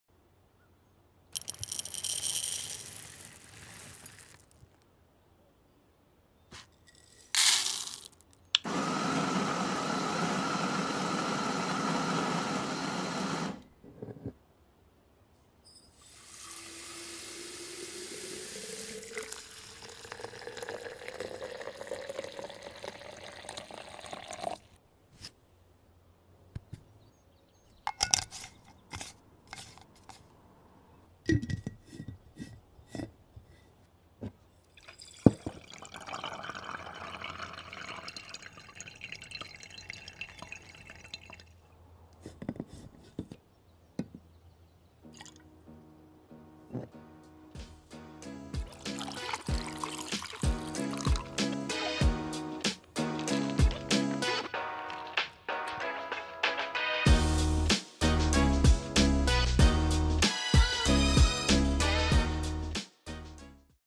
The BRUW Filter is SO Satisfying!
Making cold brew at home is super easy and super satisfying!